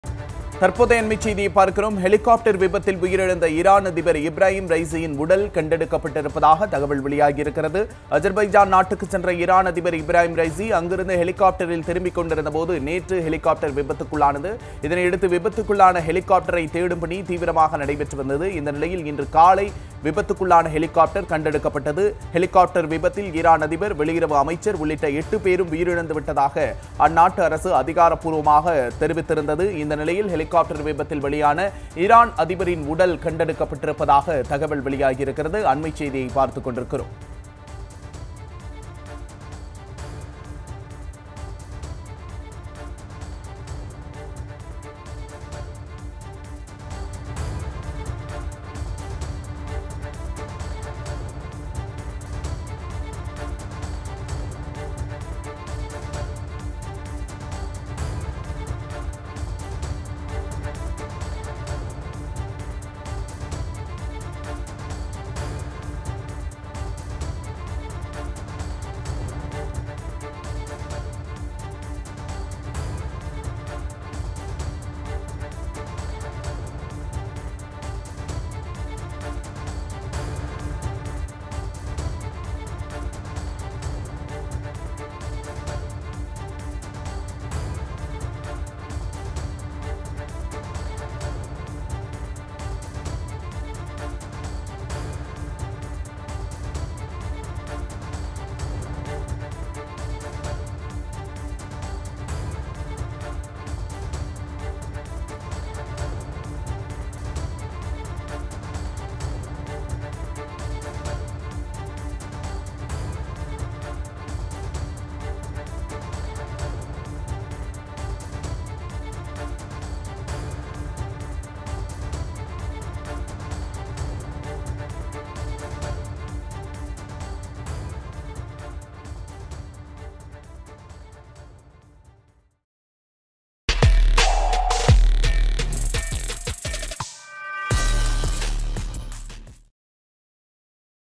# BREAKING | ஈரான் அதிபர் இப்ராஹிம் ரைசி திரும்பி வந்த ஹெலிகாப்டர் நேற்று விபத்துக்குள்ளானது
ஈரான் அதிபர் இப்ராஹிம் ரைசி திரும்பி வந்த ஹெலிகாப்டர் நேற்று விபத்துக்குள்ளானது | Kalaignar News